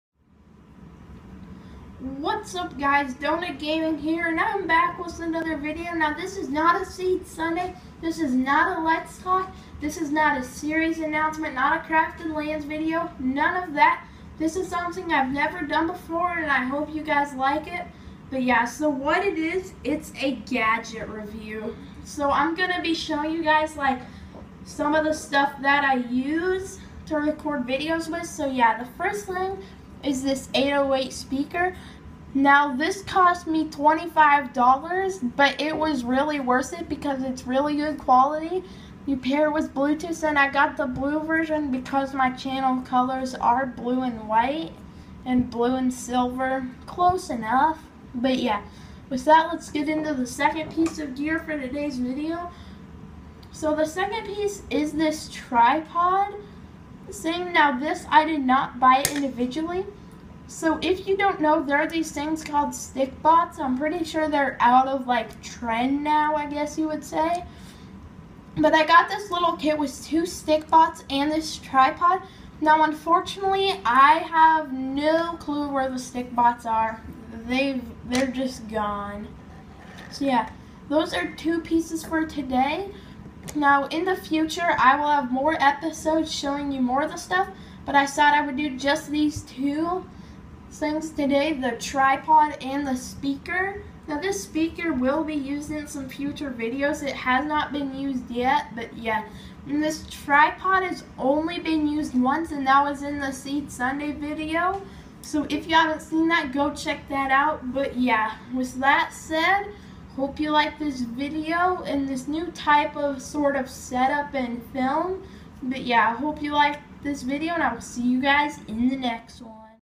Gadget Showcase
In this video I decide to change it up a bit and show you guys some of my gadgets for YouTube! This is not the first video in this mini series so be on the lookout for more. I hope you enjoy this video.